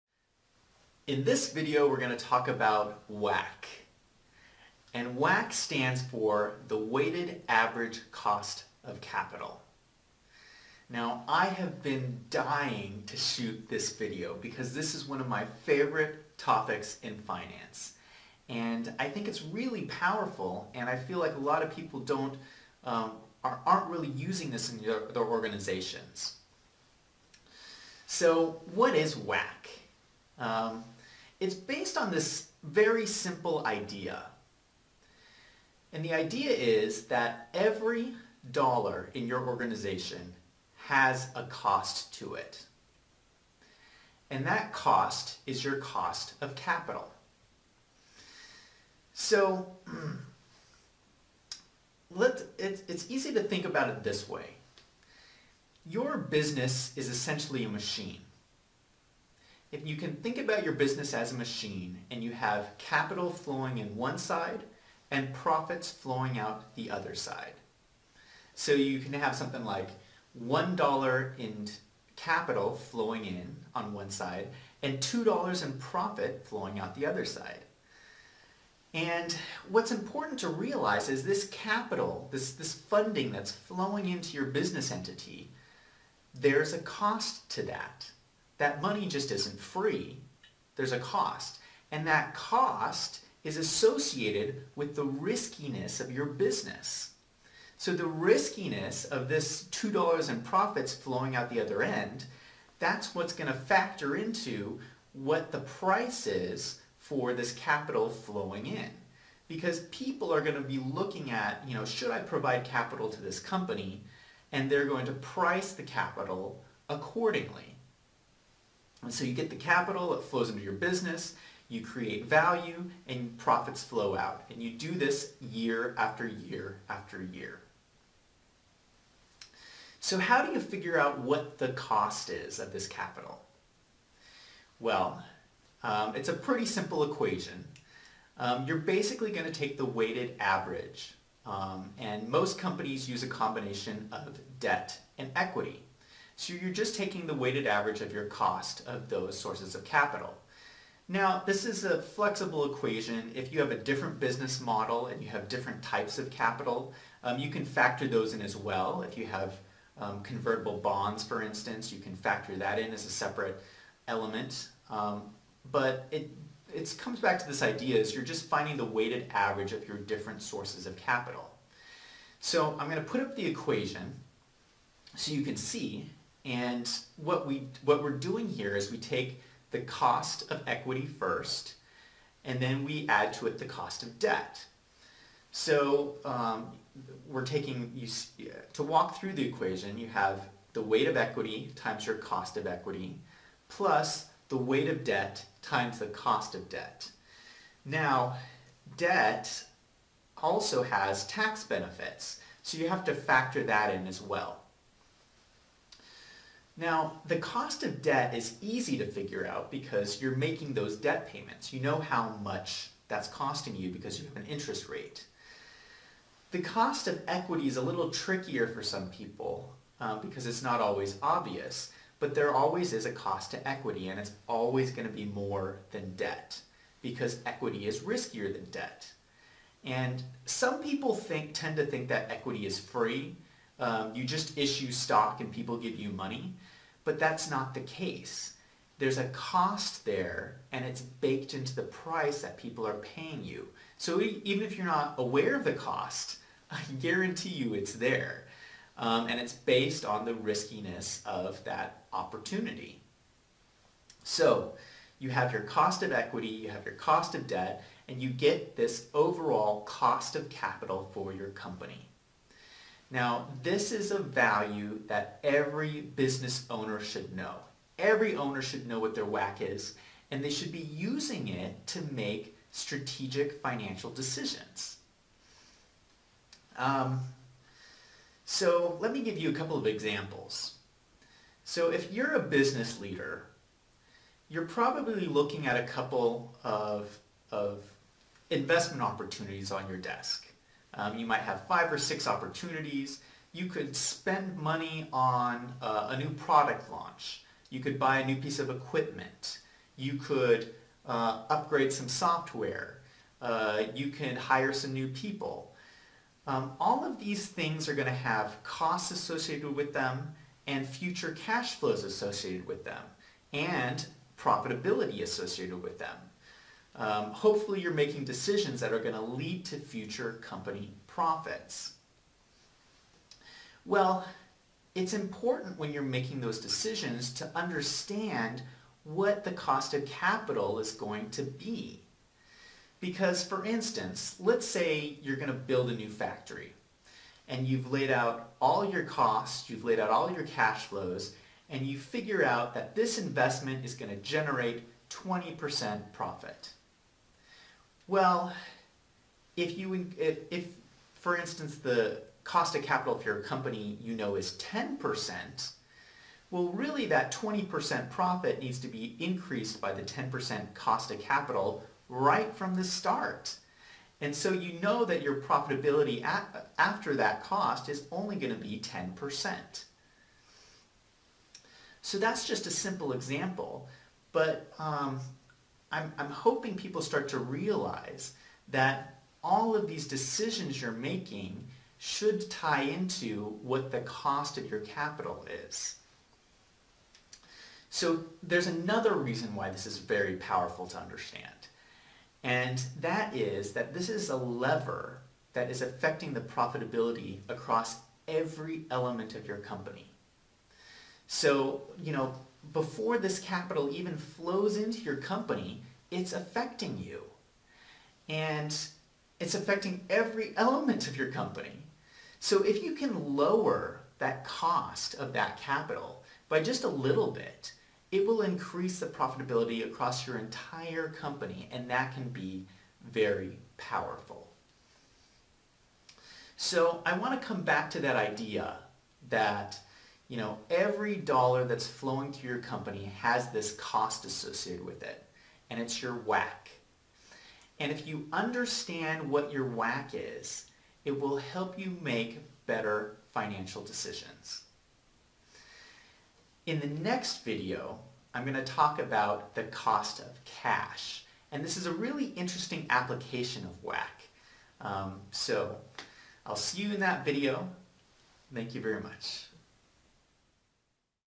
Weighted Average Cost of Capital (WACC)
ZACH DE GREGORIO, CPA

Every business owner should know their weighted average cost of capital and use it in strategic decision making. Each business functions like a machine, with capital flowing in, and profits flowing out. The capital always has a cost that is related to the riskiness of your organization. Every dollar in an organization has a cost. This video discusses how you determine the cost of capital. The equation takes the weighted average of equity (stocks) and bonds. Debt has additional tax benefits which are factored in. If you have other forms of capital, like convertible bonds, you can factor that in as well. Equity will always be more expensive than debt because it is higher risk. Cost of Capital is important when you analyze productivity. Your cost of capital becomes your hurdle rate. Potential projects are assessed based on costs and future cash flows, but the analysis should also include the cost of capital.

Neither Zach De Gregorio or Wolves and Finance Inc. shall be liable for any damages related to information in this video.